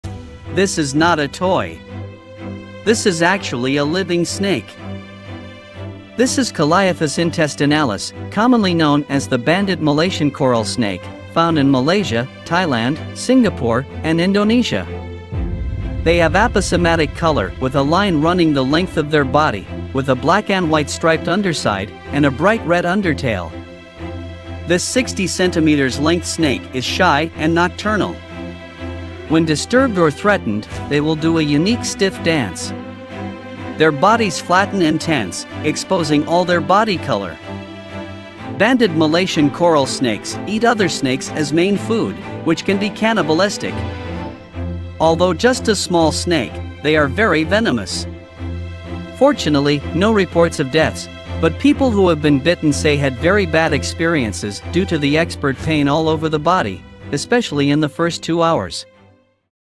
Toy-Like Banded Malaysian Coral Snake
The Banded Malayan Coral Snake (Calliophis intestinalis), is a type of highly venomous snake from the Elapidae family. This small snake has a limited distribution (endemic) in Southeast Asia. Other names are also known as the Banded Coral Snake, or Malayan Striped Coral Snake.